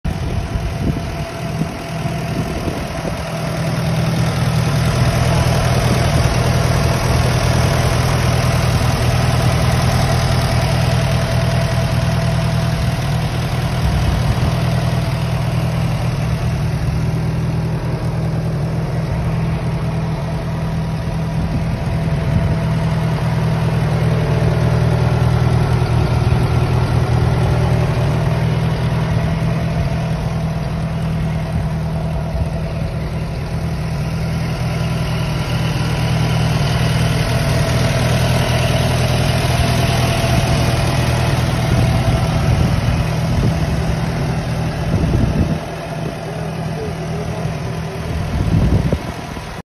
1959 INTERNATIONAL 340 For Sale
IH Farmall 340, Row Crop, runs good, stored inside [#4797]; 47-hp